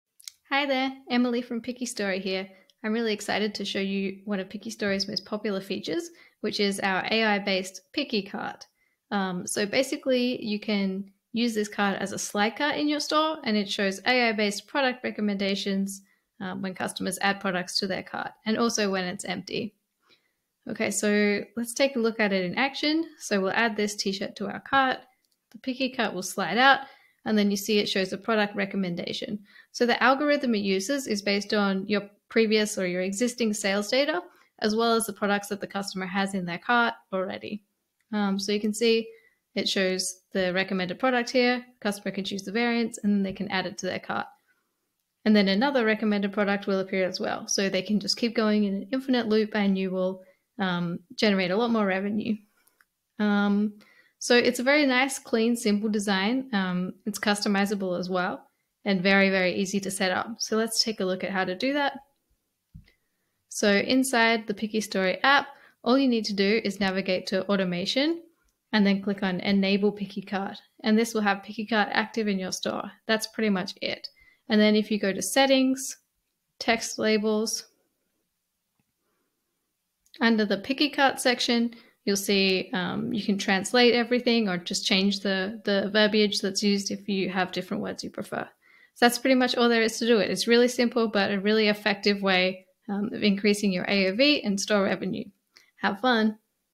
PickyCart: PickyStory's new AI-driven slide cart for Shopify stores
PickyStory now offers an amazing slide cart for Shopify with in-built, AI-based product recommendations - try free for 7 days

PickyStory’s PickyCart, is now available on all PickyStory plans for Shopify stores. We wanted to show you how to use the Shopify cart upsell PickyCart, and how it can benefit your store by increasing your conversion rate (CR) and average order value (AOV).

PickyCart can be added to any Shopify theme, and acts as a slide-out cart drawer, shown on the right-hand side of the screen when opened. PickyCart has two main sections; in the upper section, the products added to the cart are displayed. In the lower section, a “recommended for your cart” section is displayed.

PickyCart has a clean, simple design that makes it easy for the customers to see what’s already in their cart, while also highlighting “recommended products” to encourage customers to buy more, and also to help them find products they might be interested in.

PickyCart uses PickyStory’s AI-based algorithm to generate product recommendations and in-cart upsell offers. The algorithm uses your existing sales data to select products that are commonly purchased together, in order to offer the product combination in the cart that’s most likely to result in a sale.